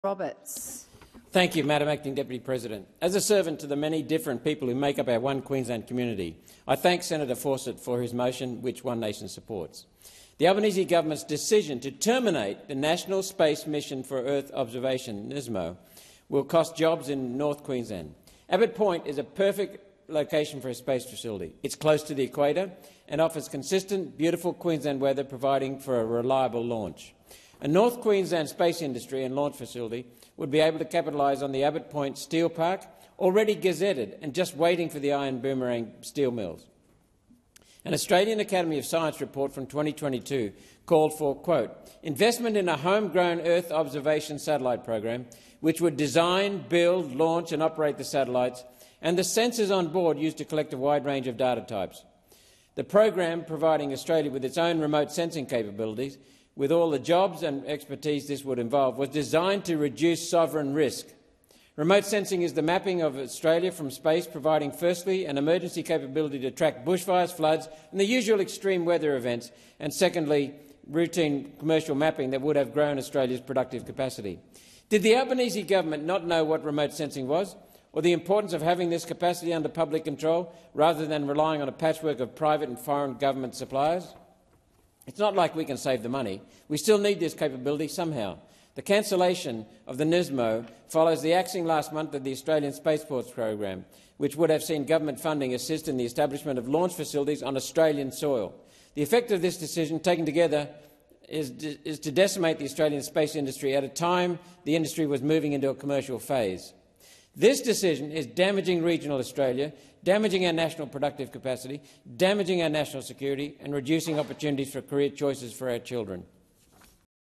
The Labor Government's War on Space
I spoke in support of a motion to protect Australia's space program. The Albanese government's decision to terminate the national space mission for Earth observation (NISMO) is shortsighted and false economy.

Investment in a homegrown space program designing, building and launching satellites was a recommendation by the Australian Academy of Science in a 2022 report. Apart from the career opportunities and jobs the program would have offered, it also provided national security.

It would provide Australia with its own remote sensing capabilities and reduce sovereign risk. It would give us the ability to respond to emergencies and track bushfires, floods and extreme weather events.

Does PM Albanese not understand remote sensing?

The cancellation of NISMO follows last month's axing of the Australian Spaceports Program, which would have seen government funding assist the establishment of launch facilities on Australian soil.

This is not about saving money — we still need this capability somehow. It would be far better to have the capability under public control rather than relying on a patchwork of private and foreign suppliers.